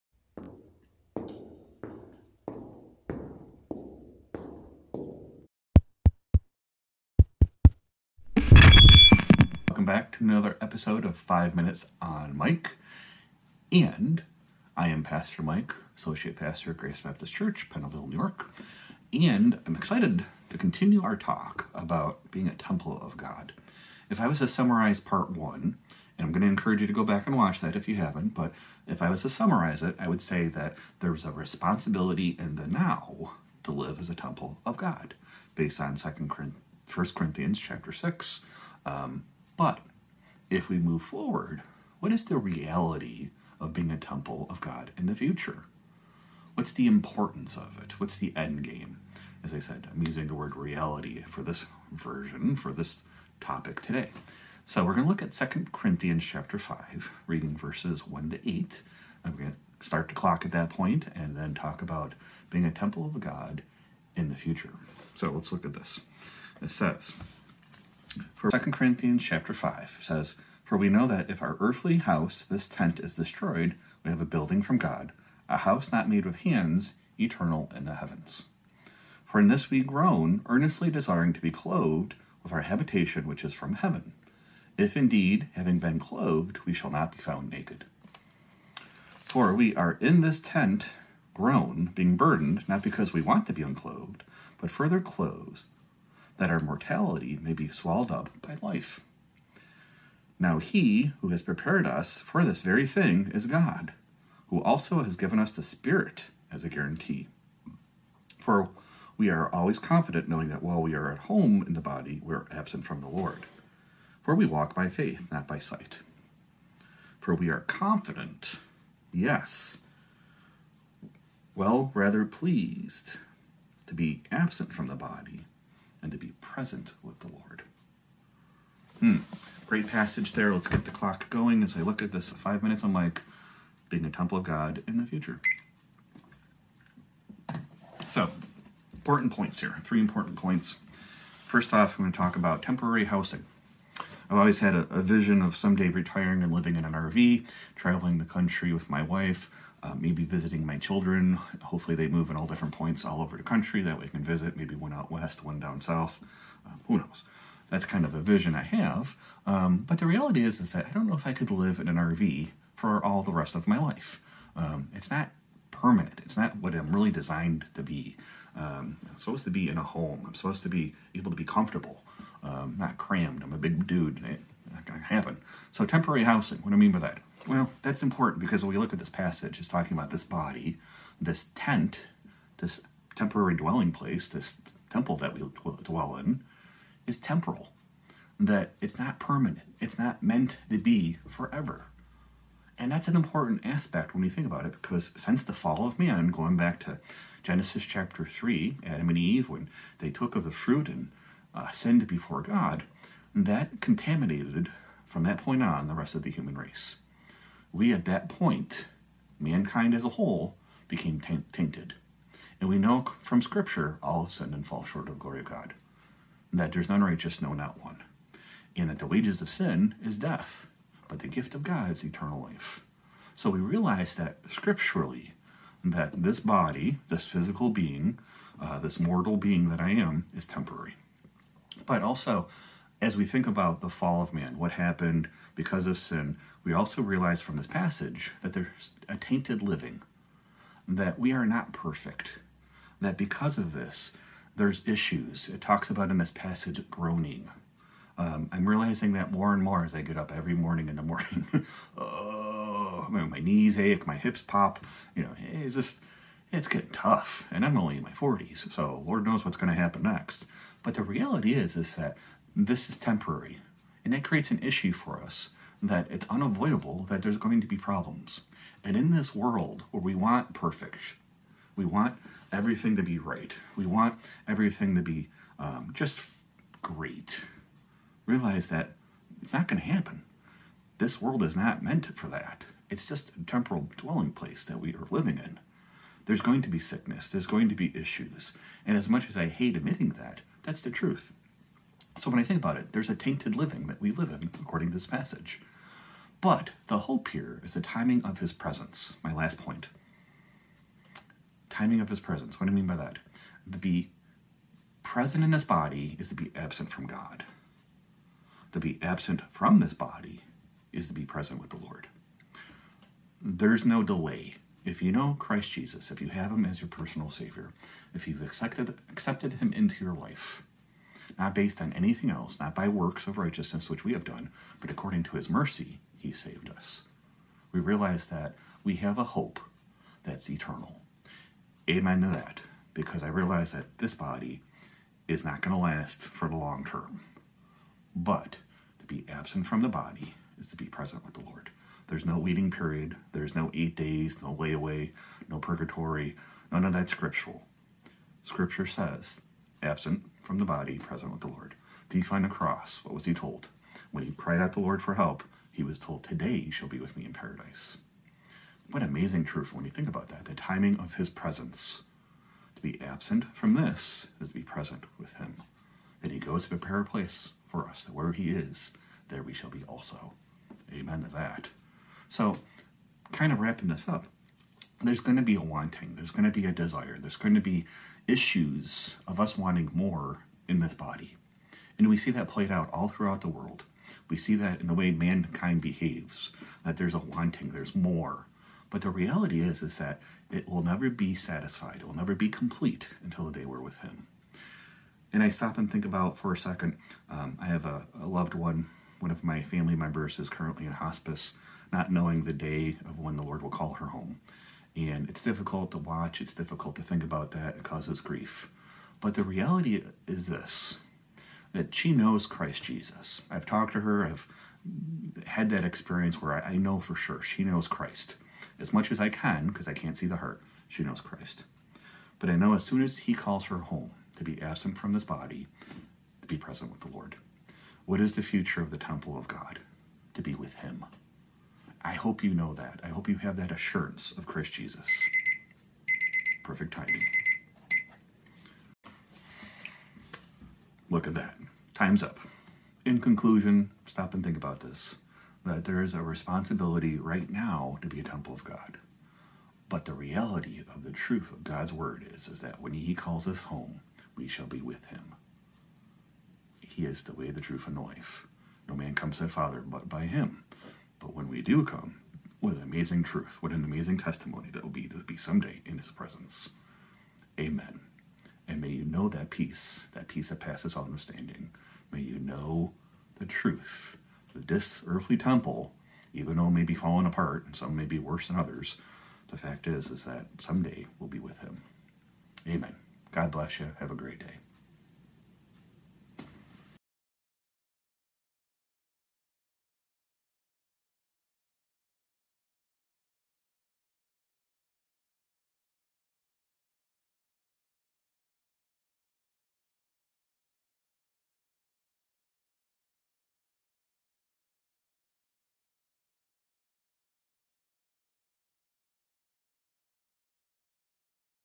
Being a Temple of God - part 2.mp4
What does being a temple of God mean to you in the scope of eternity? We struggle in this world because we are not of this world anymore...